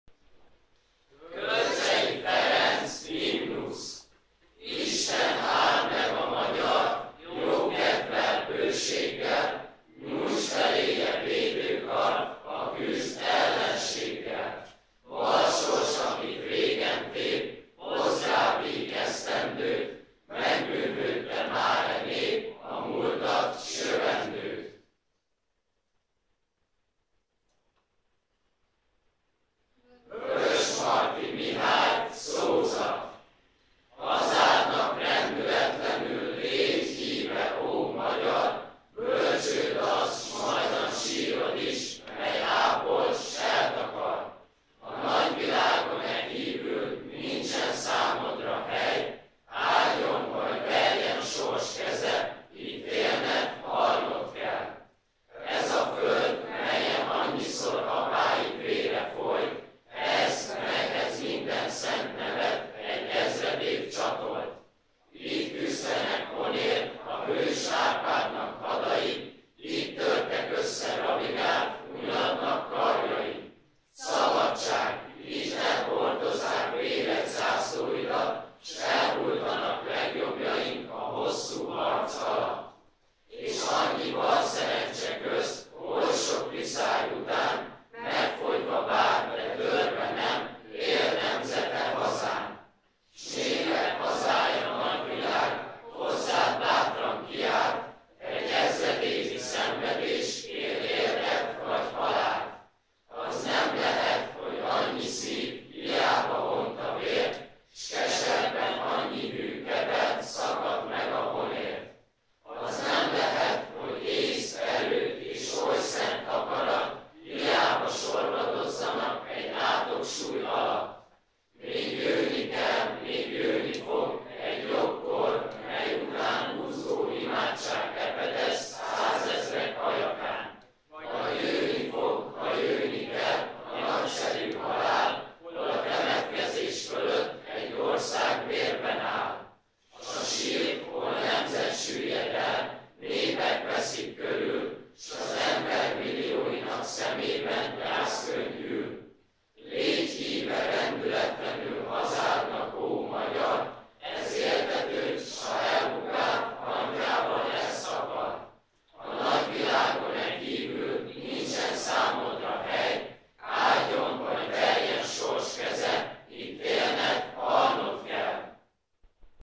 Együtt Szaval a Nemzet_Orosháza-Tisza Kálmán Szakképző Iskola
Nem regisztrált résztvevőként szaval a diákság.